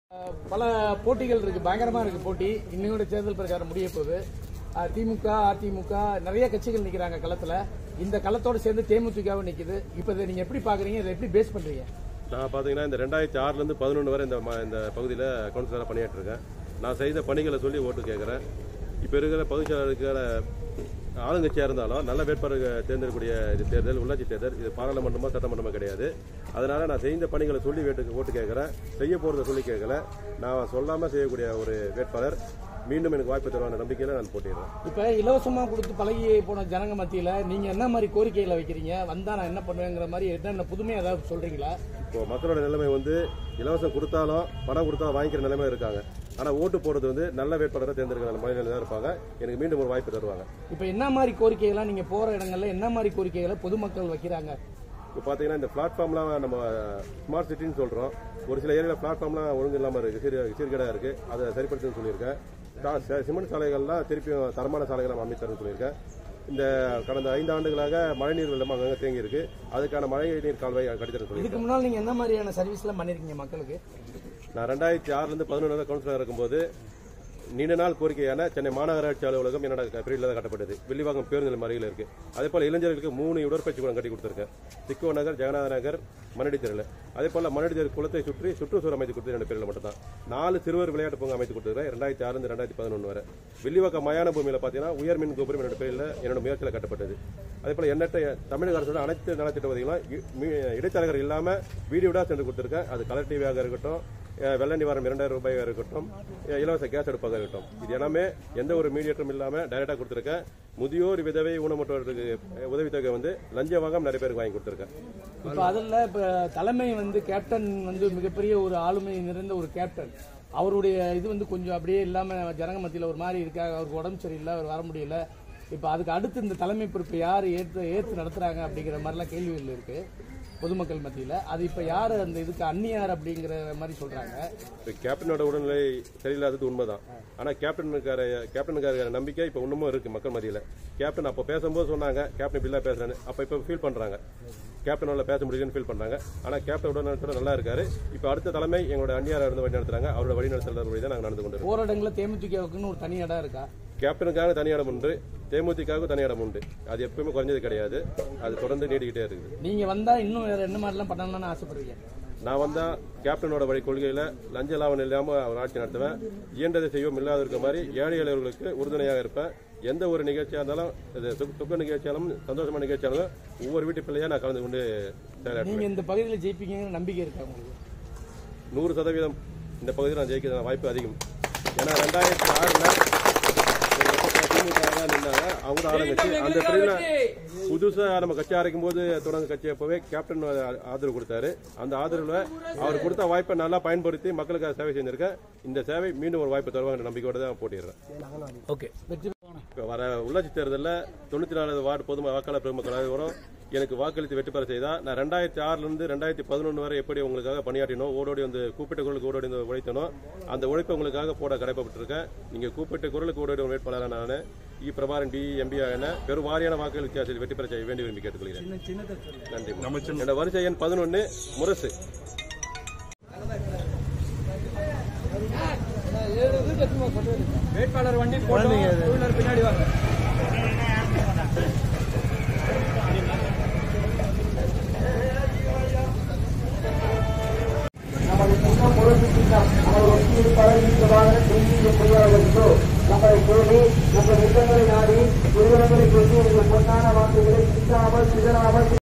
94 வது வார்டு கவுன்சிலர் பதவிக்கு போட்டியிடும் E.பிரபாகரன் சிறப்பு பேட்டி
94 வது வார்டு கவுன்சிலர் பதவிக்கு போட்டியிடும் E.பிரபாகரன் சிறப்பு பேட்டி